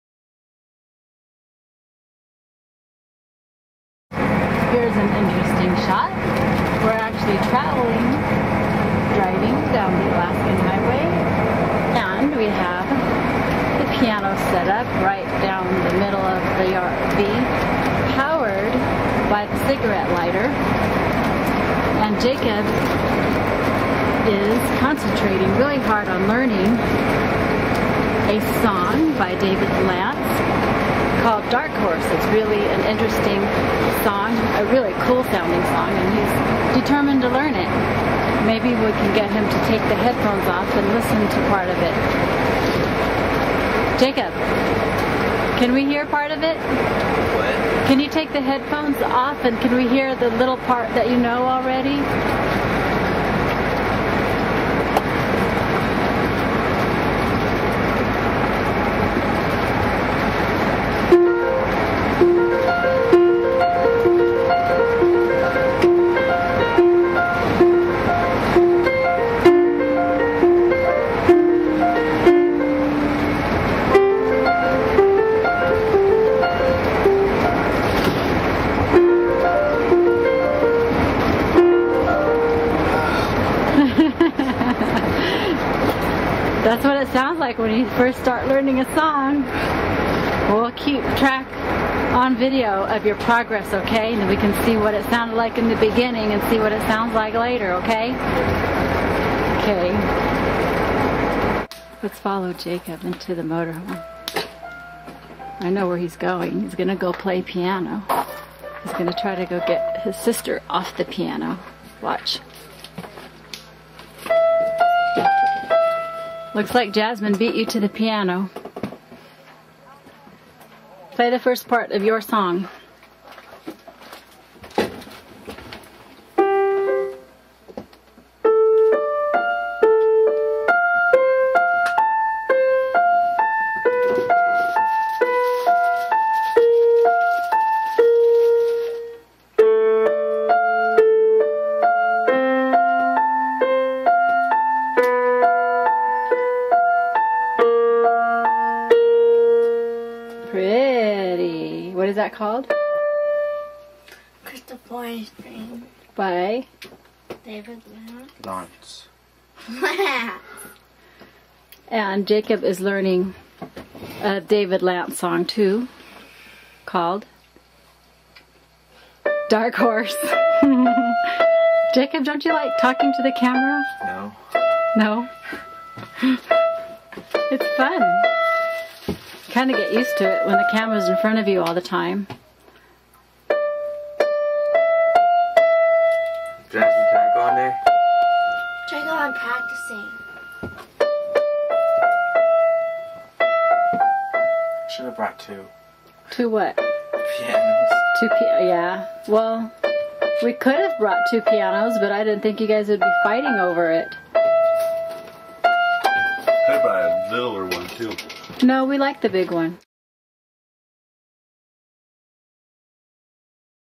Jacob and Jasmine Play Piano in the RV
Alaska Trip 2009
Steeple Family